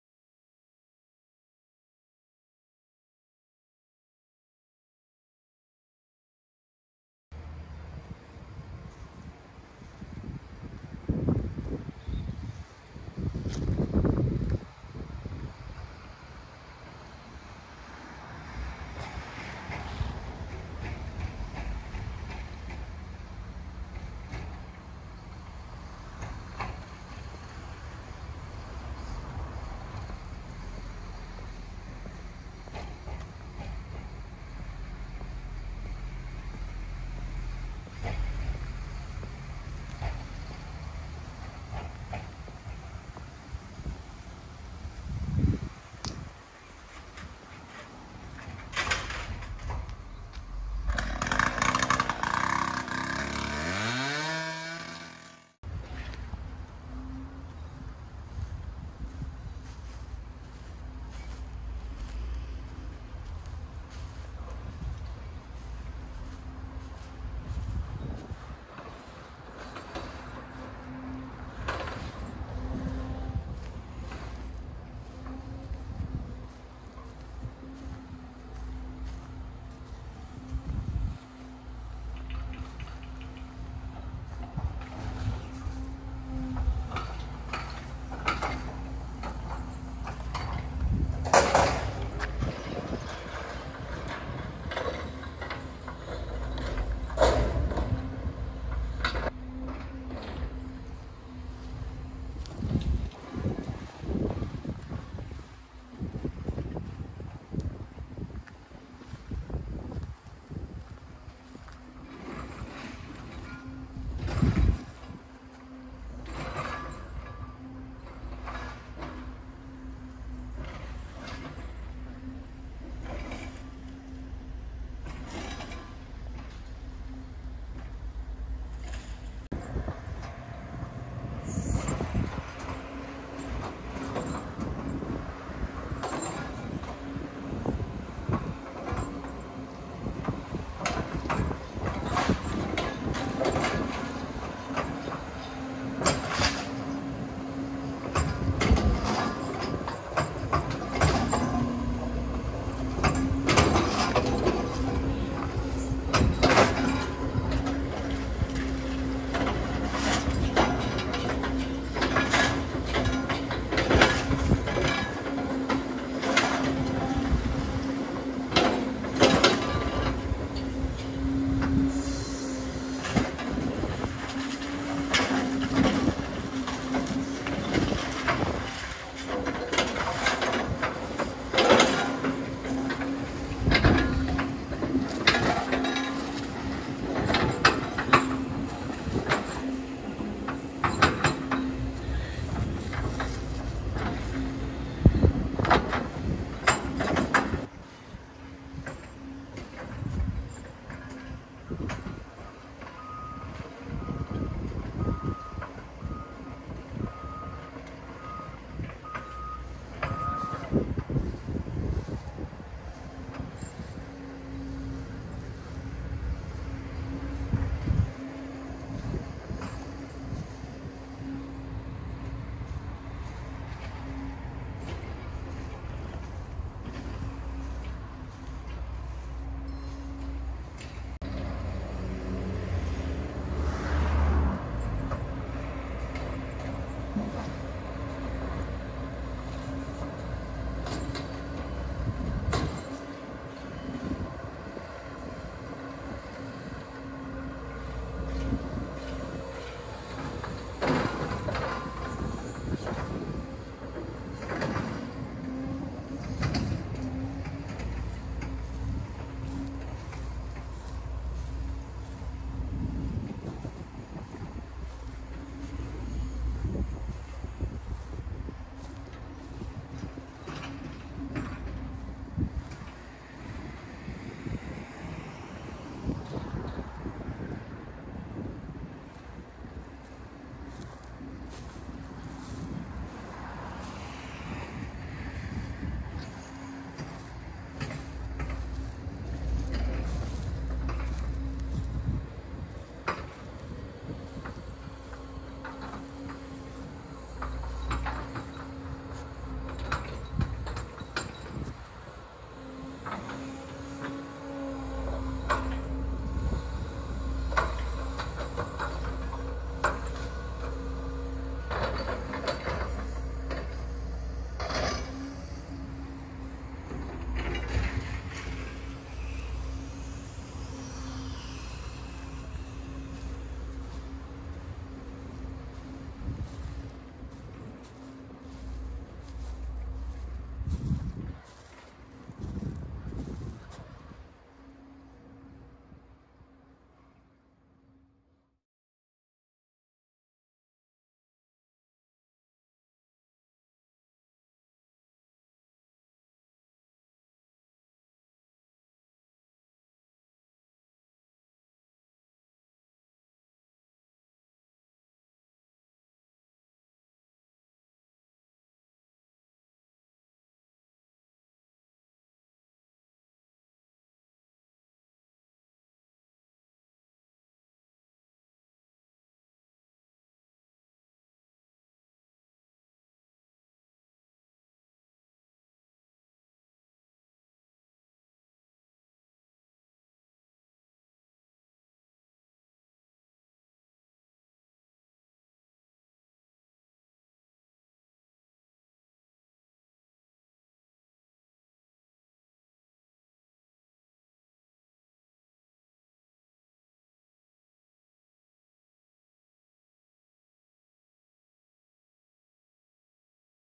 Ohio st. Apartments Demolition San Diego [4k]
Ohio st Demolition in San Diego. Photo of Coming Soon Artist's Rendition. Shot on iPhone 11 Pro 4k 60fps - edited on Davinci Resolve. No color grading, exception saturation on JPG photo at end.